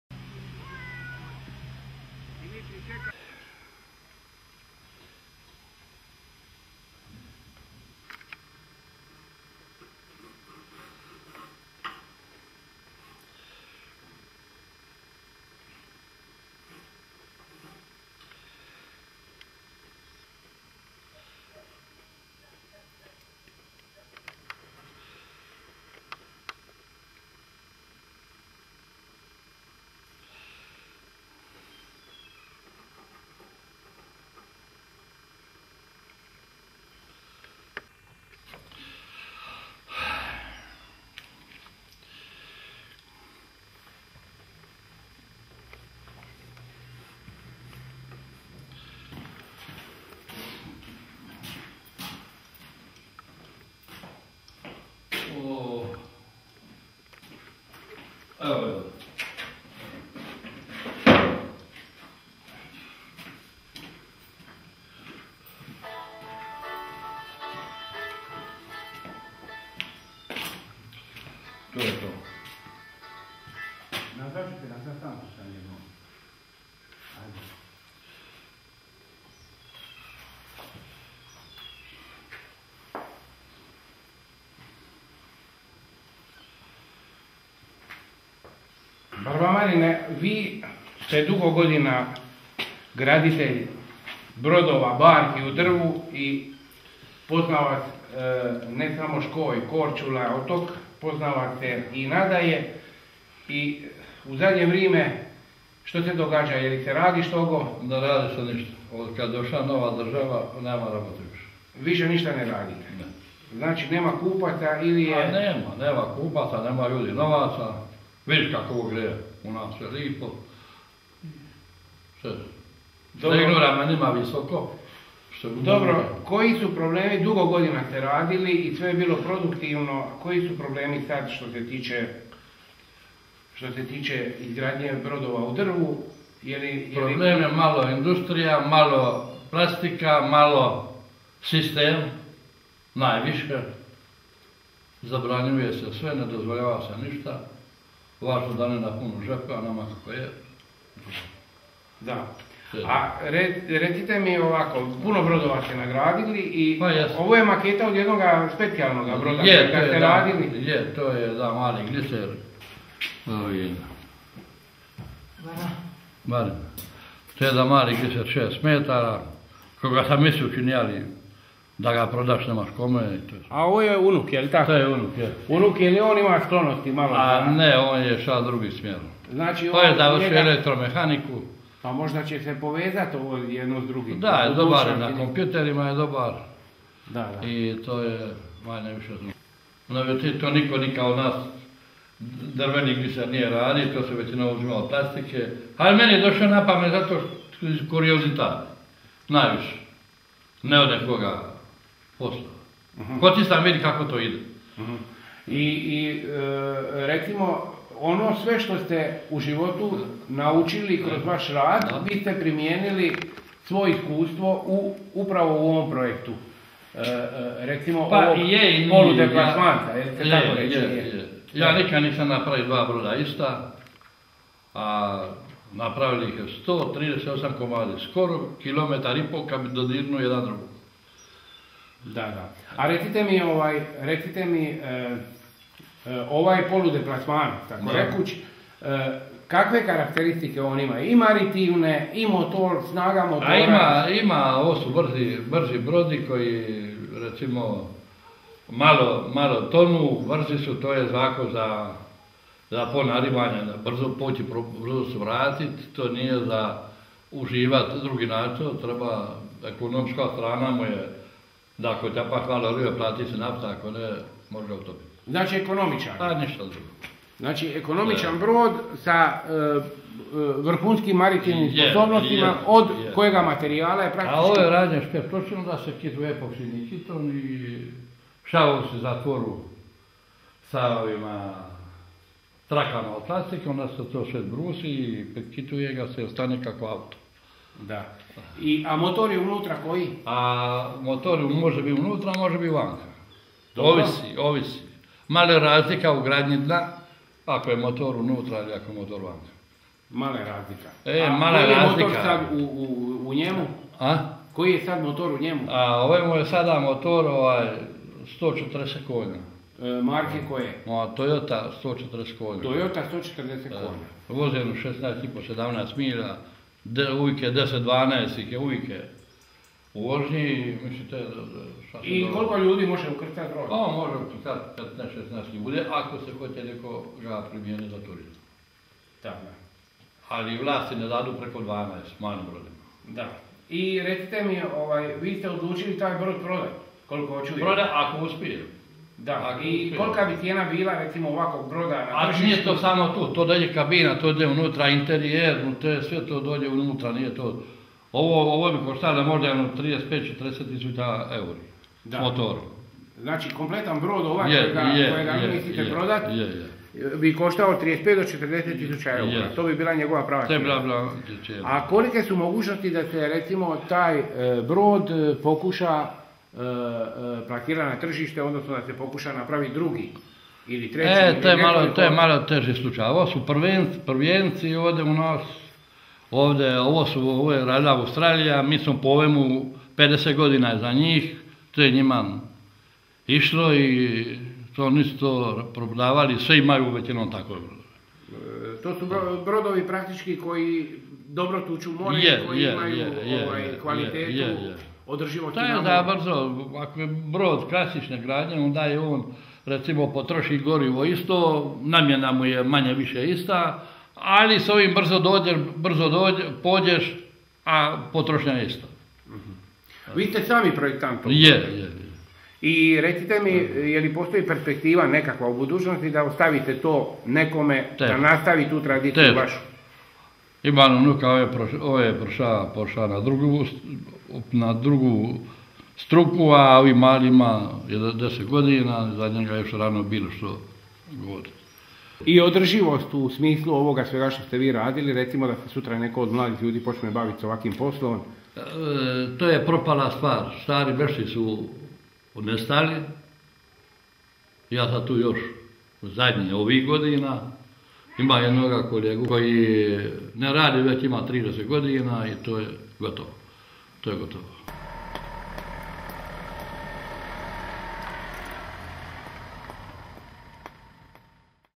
Barba Marin - Model drvenog glisera (MarcoPoloTV Officia Video) [HD]
Barba Marin iz Žrnovske Banje ispričati će
nam o izgradnji drvenog glisera.
©Powered by: Studio Marco Polo
Please report any errors/violations you know of.